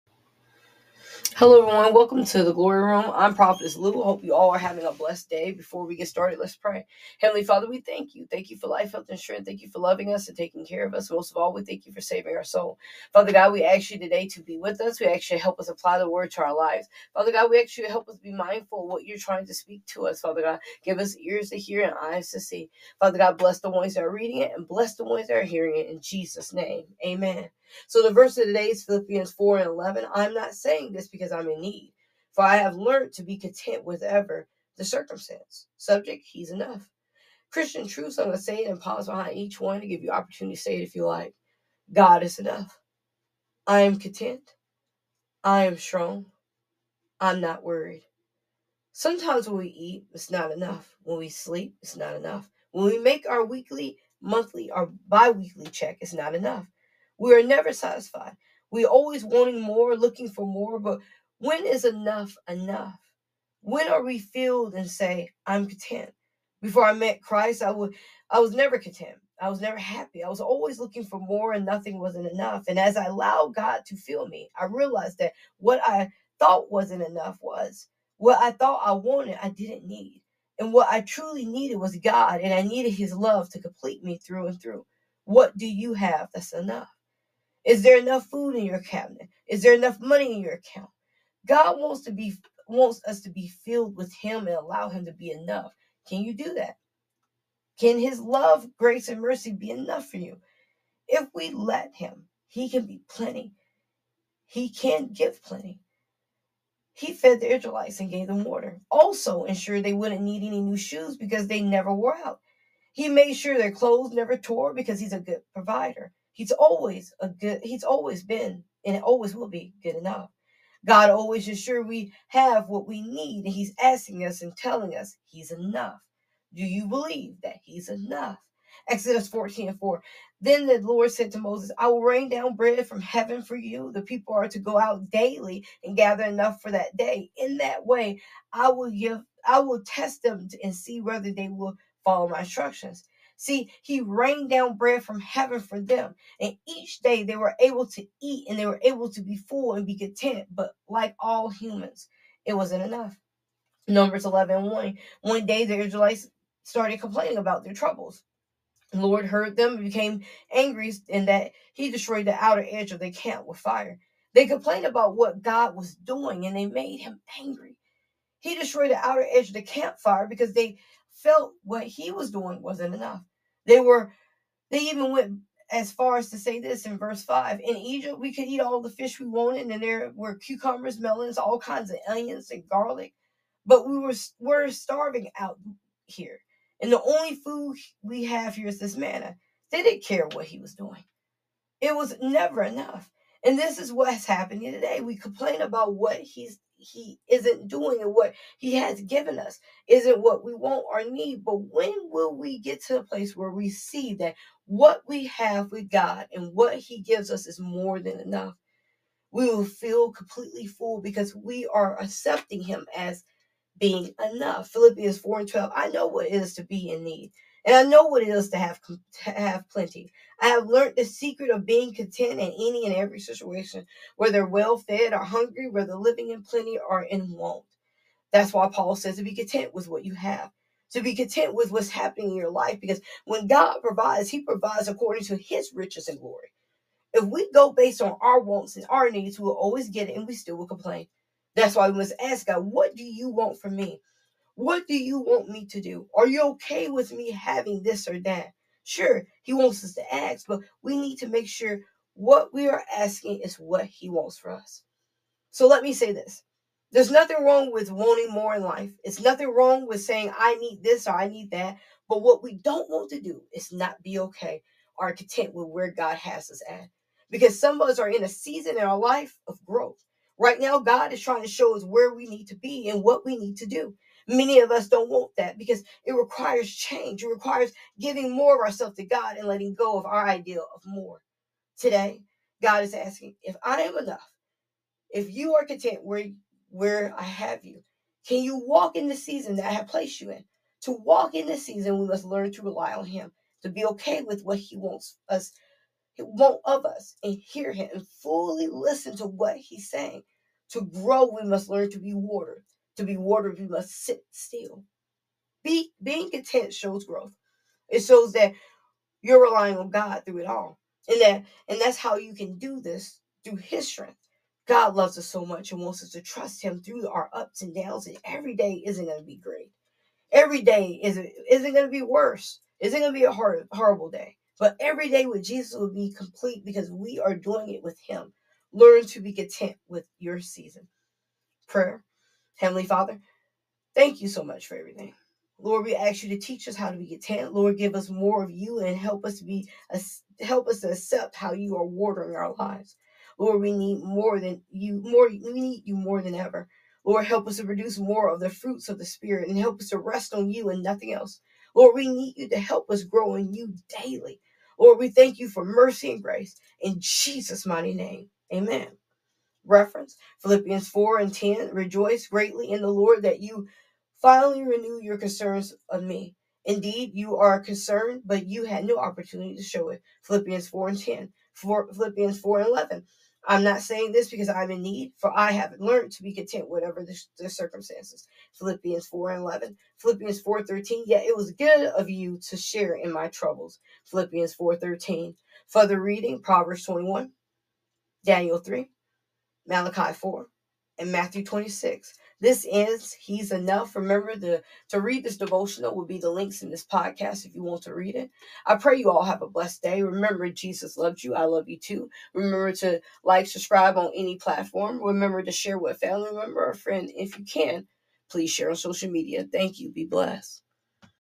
He's Enough
MEMORY VERSE OF THE WEEK: + 2 Chronicles 15:7 But as for you, be strong and do not give up, for your work will be rewarded.”

VERSE OF THE DAY : + Philippians 4:11 I am not saying this because I am in need, for I have learned to be content whatever the circumstances.

REFERENCES
+ Philippians 4:10 rejoiced greatly in the Lord that you finally renewed your concern for me. Indeed, you were concerned, but you had no opportunity to show it.

+ Philippians 4:11 I am not saying this because I am in need, for I have learned to be content whatever the circumstances.

+ Philippians 4:13 Yet it was good of you to share in my troubles.

FURTHER READINGS
Proverbs 21
Daniel 3
Malachi 4
Matthew 26